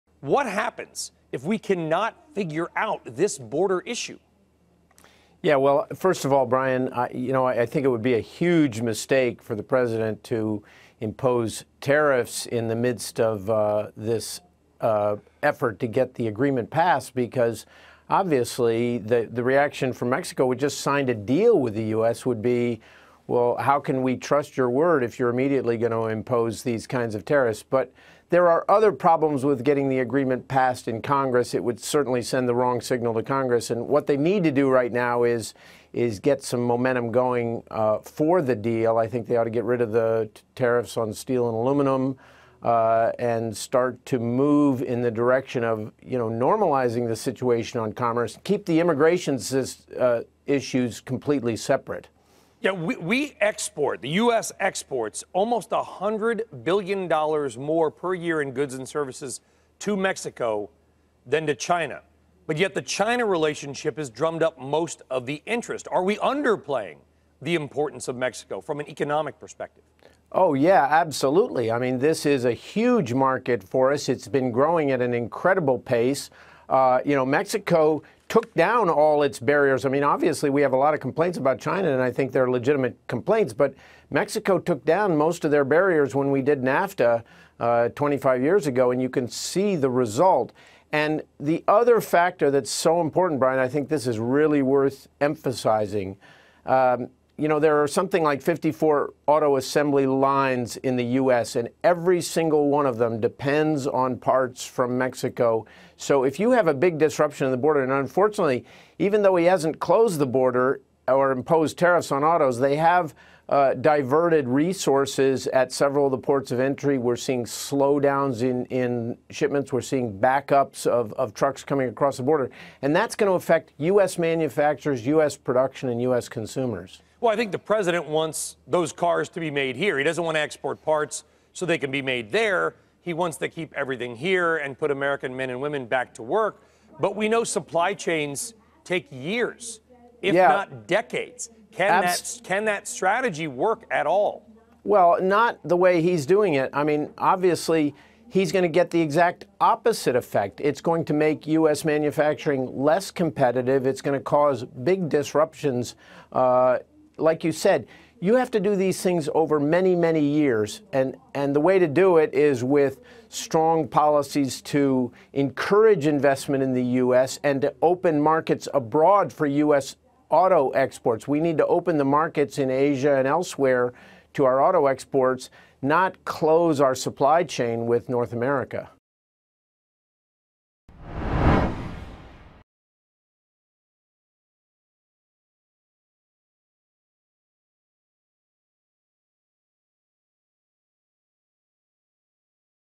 Rufus Yerxa on the USMCA
Rufus Yerxa, National Foreign Trade Council President, talks about the ongoing trade situation with Mexico.